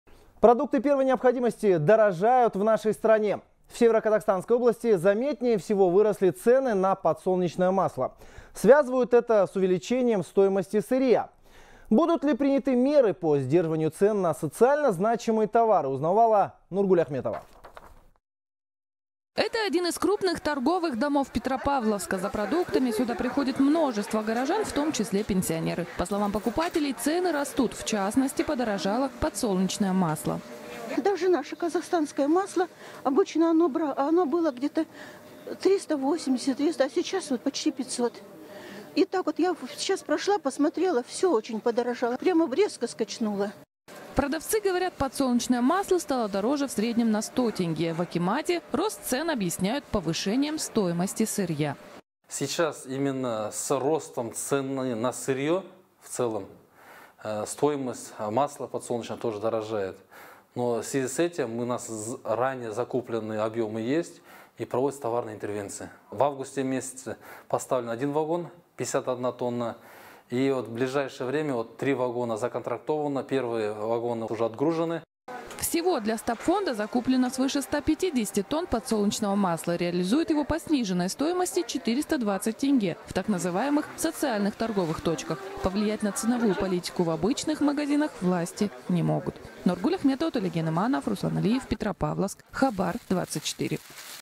Продукты первой необходимости дорожают в Казахстане
Продукты первой необходимости дорожают в Казахстане. В Северо-Казахстанской области заметнее всего выросли цены на подсолнечное масло, передает корреспондент «Хабар 24».

Связывают это с увеличением стоимости сырья. Будут ли приняты меры по сдерживанию цен на социально-значимые товары, узнавала Нургуль Ахметова.

Это один из крупных торговых домов Петропавловска. За продуктами сюда приходит множество горожан, в том числе – пенсионеры. По словам покупателей, цены растут, в частности подорожало подсолнечное масло.